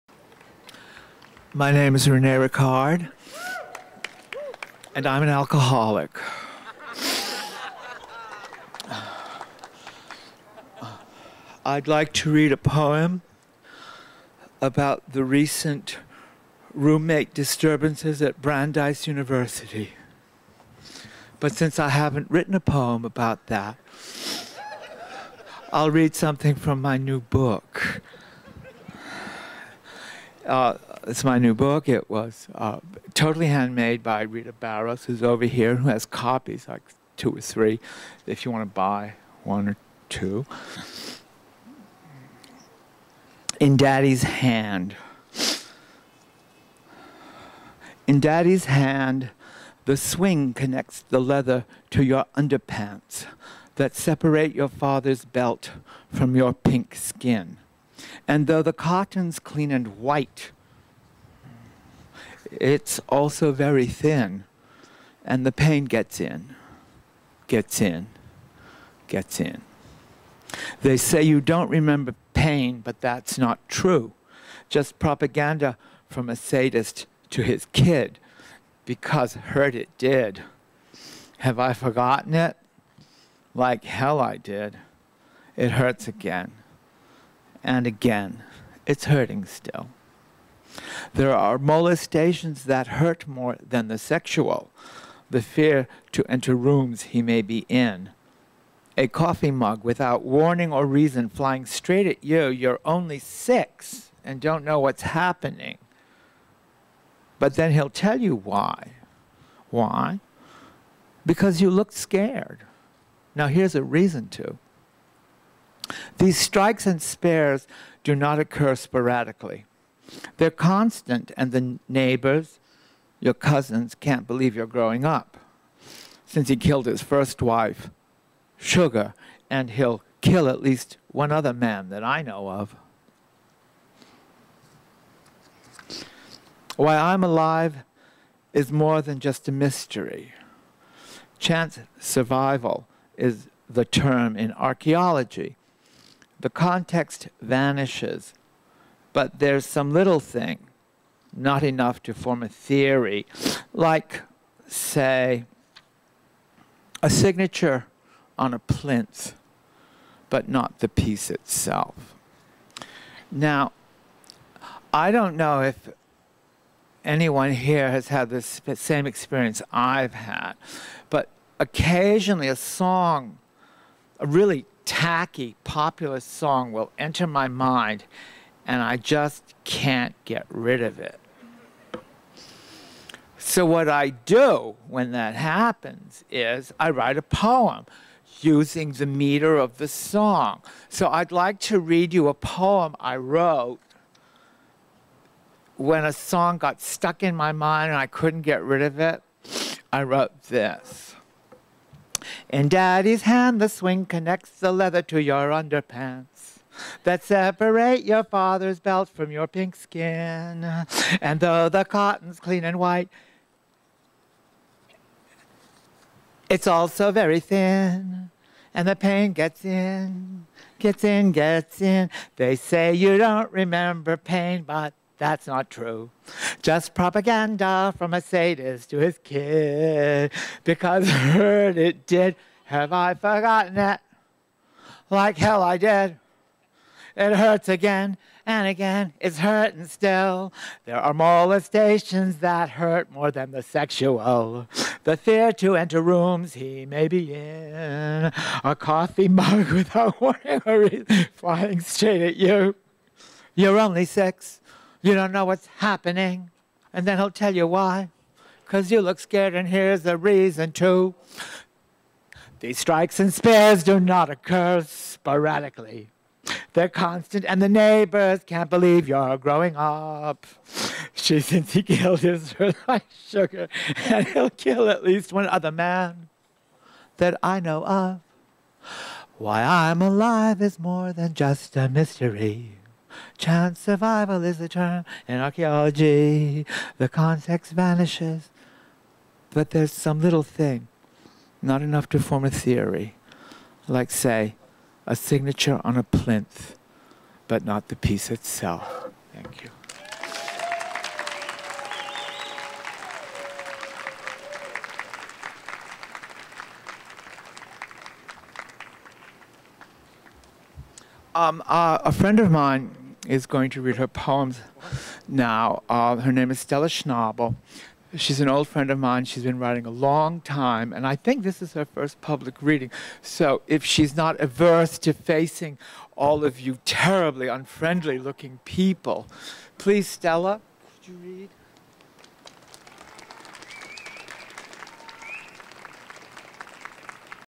OIL KILLS POETS SPILL - Rene Ricard cam1
Oil Kills Poets Spill reading in New York marble cemetery.